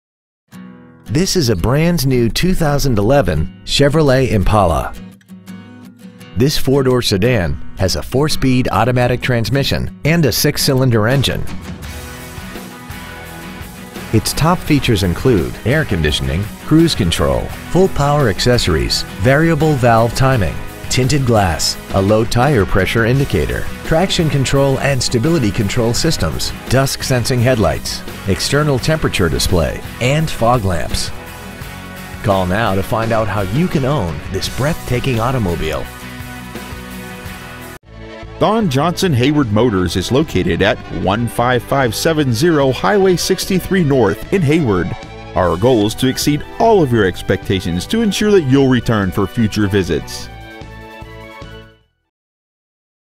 2011 Chevrolet Impala Hayward WI
We've been honored to serve the Hayward WI area , we promise that your experience at our dealership will exceed your expectations !

 Year : 2011
 Make : Chevrolet
 Model : Impala
 Engine : 6 Cylinder
 Trans . : Automatic 4-Speed
 Exterior : Red Jewel Tintcoat
 Miles : 10
 Interior : Neutral w/Leather-Appointed Seating
 Stock : H11055

 2011 Chevrolet Impala Hayward WI